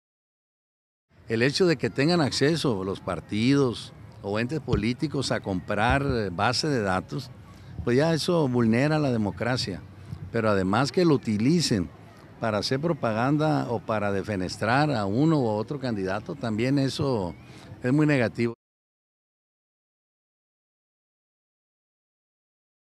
RECHAZA RECTOR ACCESO A DATOS PERSONALES POR PARTE DE PARTIDOS POLITICOS
Advierte que el acceso a datos personales de parte de los partidos politicos, vulneran la democracia